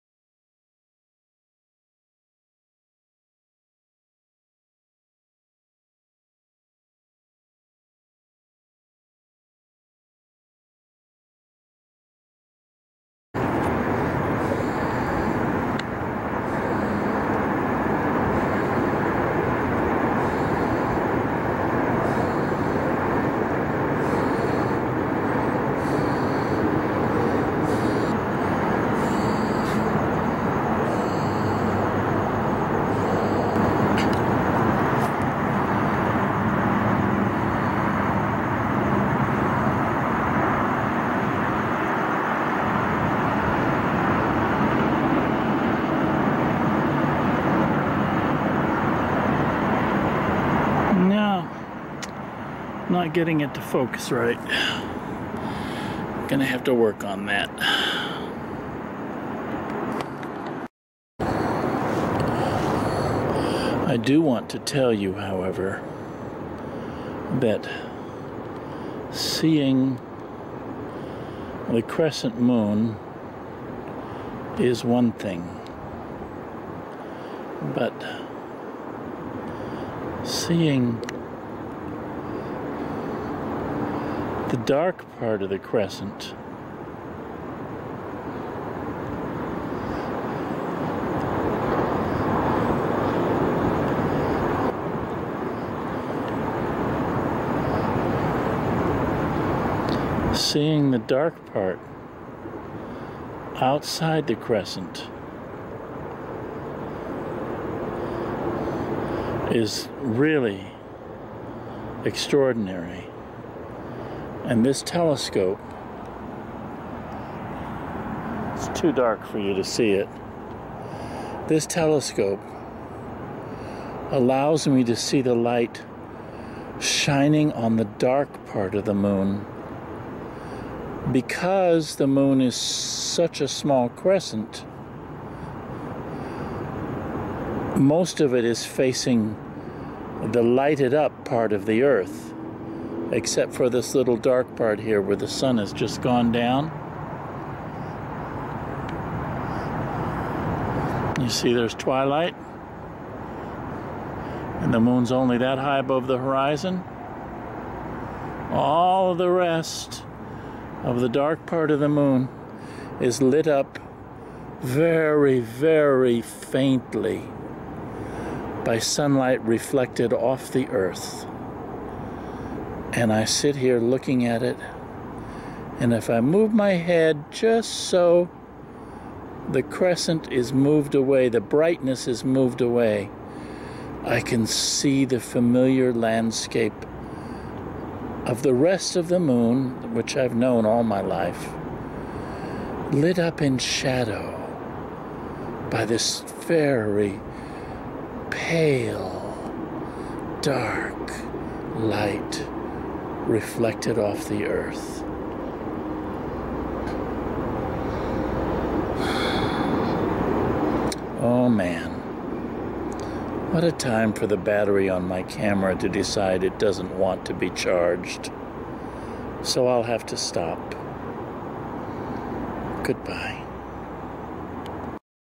camera at the moon fail
I look at the crescent moon this evening and try to point my video camera into the eyepiece with virtually NO success whatsoever but I had a good time anyway and then I tell you why.